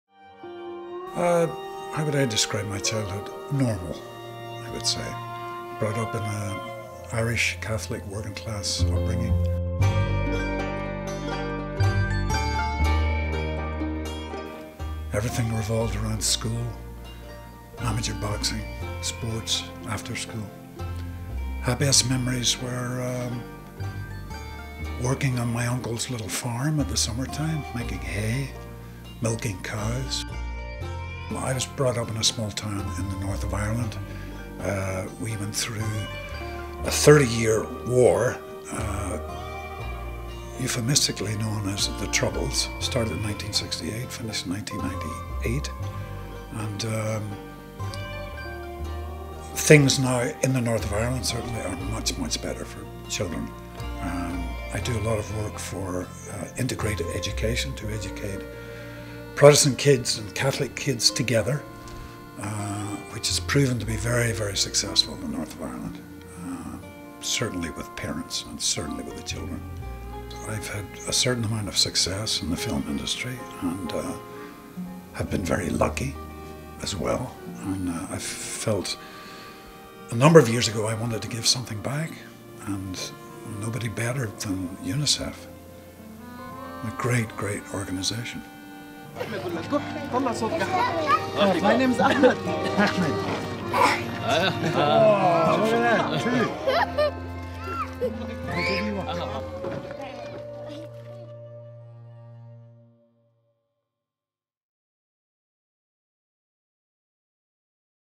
Liam Neeson’s childhood | UNICEF
Liam Neeson shares his happiest childhood memories and why he’s standing up for children’s rights.